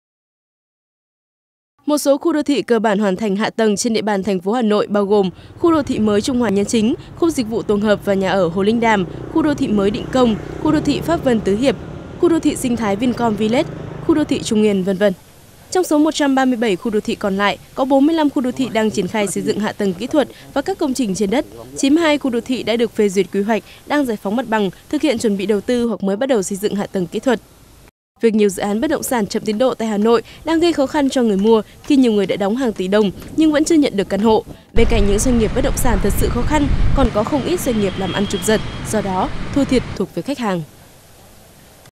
VTC14_Hà Nội còn gần 140 khu đô thị dở dang
Trong tổng số 152 khu đô thị trên địa bàn Thủ đô, mới có 15 khu đô thị cơ bản hoàn thành hạ tầng và đi vào sử dụng. 137 khu đô thị còn lại vẫn trong tình trạng dở dang.